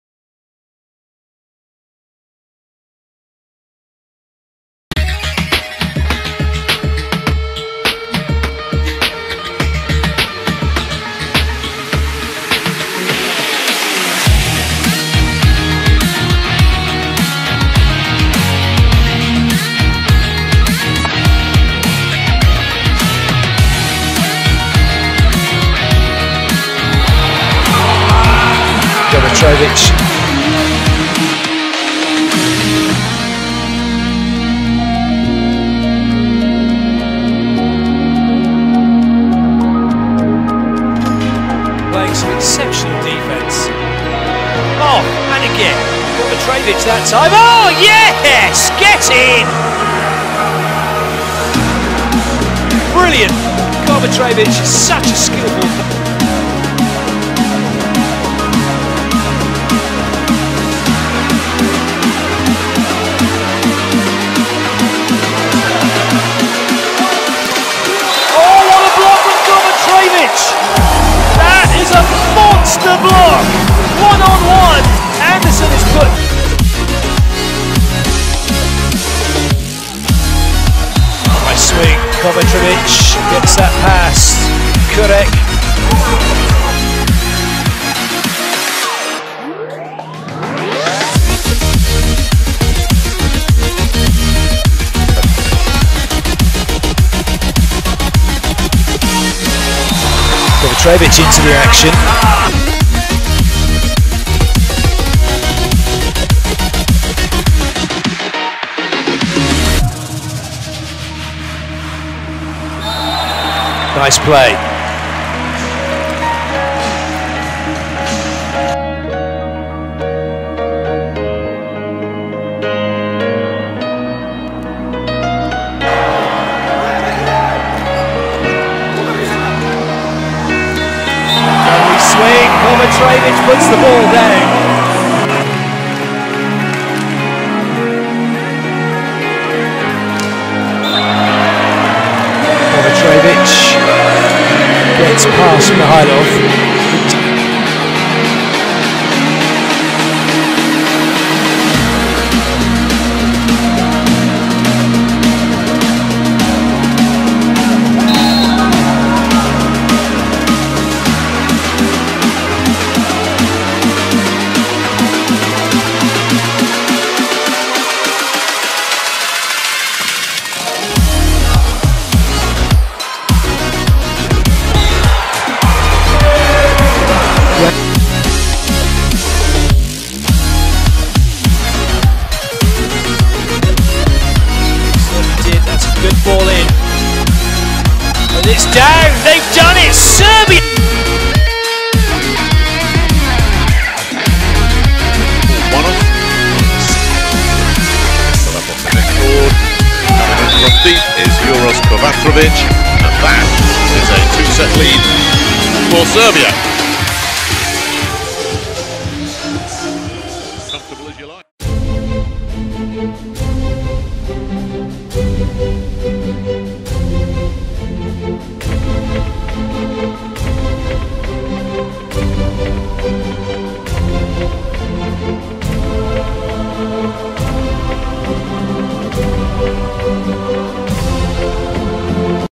Uros Kovacevic | The Best Left Handed Volleyball Player | FIVB Mens WCH 2018
Uroš Kovačević (Serbian Cyrillic: Урош Ковачевић, born 6 May 1993) is a Serbian volleyball player, a member of Serbia men's national volleyball team and Italian club Diatec Trentino, European Champion 2011

2011 CEV U19 European Championship - Most Valuable Player
2011 FIVB U19 World Championship - Most Valuable Player
2011 Award of Olympic Committee - Young Athlete of The Year
2011 Young Athlete of The Year by DSL Sport
2013 FIVB U23 World Championship - Best Outside Spiker
2016 Memorial of Hubert Jerzy Wagner - Best Outside Spiker
2016 Asian Club Championship - Best Outside Spiker
2018 FIVB Club World Championship – Best Outside Spiker